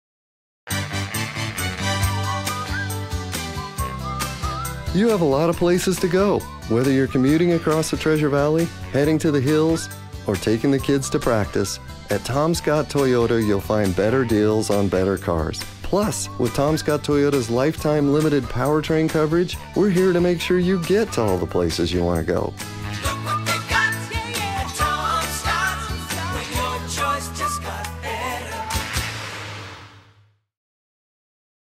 Tom Scott Toyota: Lots of Places to Go TV Spot
"You have a lot of places to go, whether you're commuting across the Treasure Valley, heading to the hills, or taking the kids to practice. At Tom Scott Toyota, you'll find better deals on better cars. Plus, with Tom Scott Toyota's lifetime limited powertrain coverage, we're here to make sure you get to all of the places you want to go."